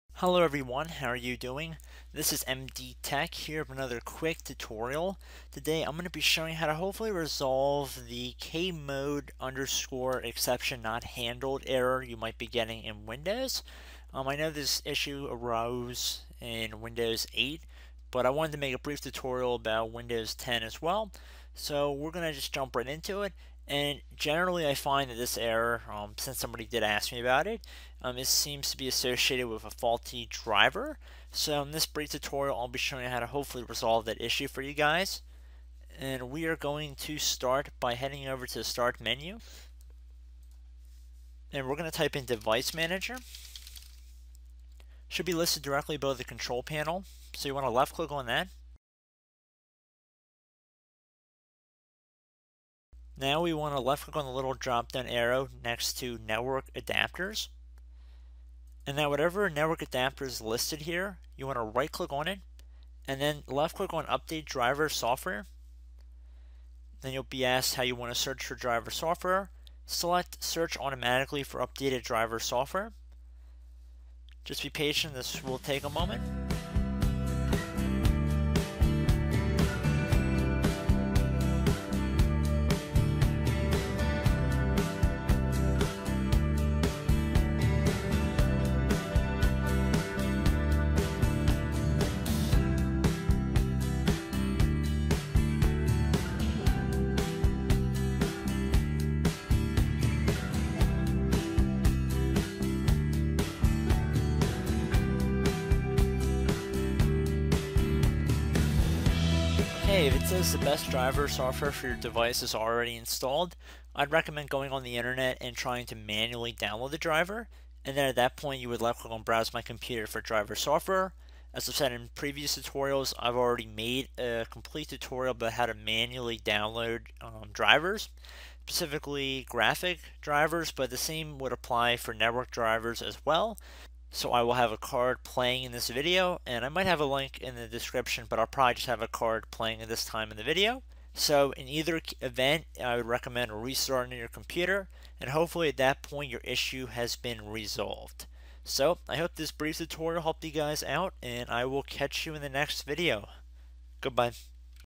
How To Fix KMODE EXCEPTION NOT HANDLED In Windows 10
Fix system thread exception not handled or  how to resolve a common blue screen on Windows 10.

KMODE_EXCEPTION_NOT_HANDLED are two blue screen errors that will be addressed in this tutorial.

The kmode exception not handled error is likely stemming from a faulty driver. This tutorial will show you how to start your Windows 7 or Windows 10 computer into safe mode to uninstall or update the driver causing the problem.